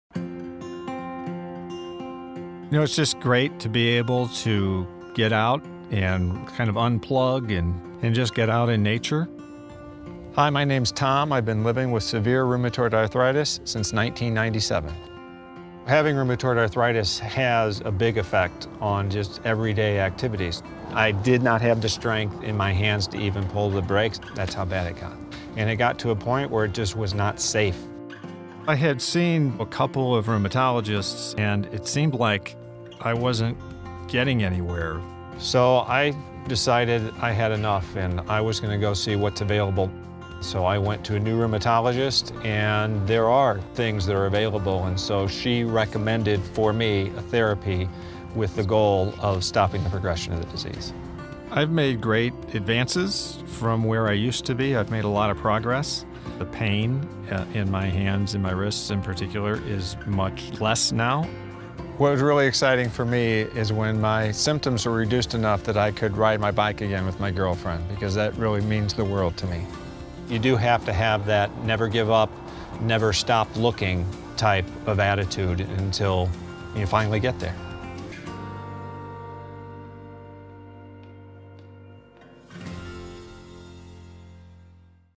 Tom’s Rheumatoid Arthritis Story | Rheum for More
Hear Tom talk about his experience with rheumatoid arthritis (RA) and how he got back to a more active life with the right treatment plan.

After seeing a couple of rheumatologists and feeling like he wasn’t getting anywhere, Tom decided it was time to find a new rheumatologist.

Learn how Tom partnered with his rheumatologist to find an RA treatment plan that helped him meet his personal goals of exercising with RA.

Listen to Tom talk about how having a “never give up” attitude can motivate you to talk to a doctor about reaching your goals.